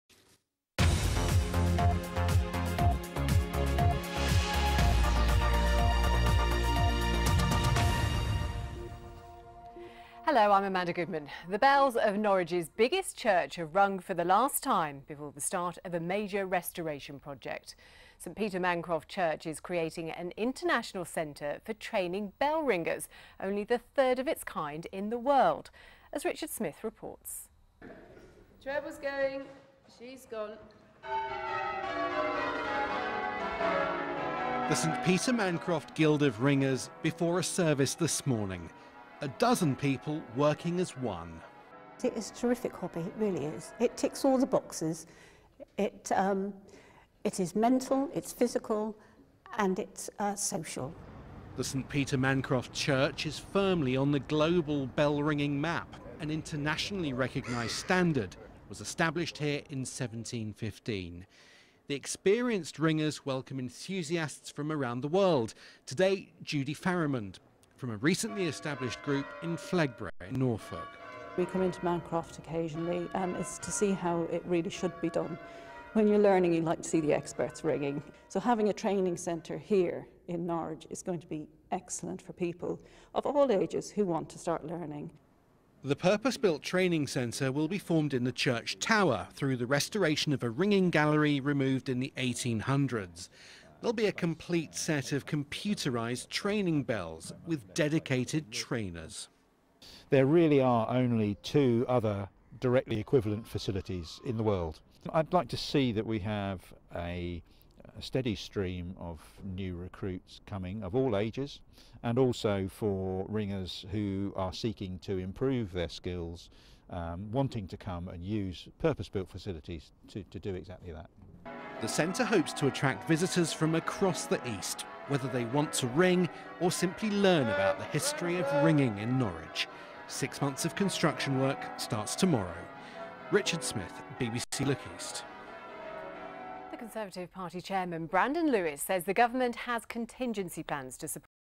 Mancroft 300 Project - BBC 14 January 2018
Coverage on the St Peter Mancroft 300 bell project broadcast on BBC East on 14 January 2018.  Courtesy of BBC East.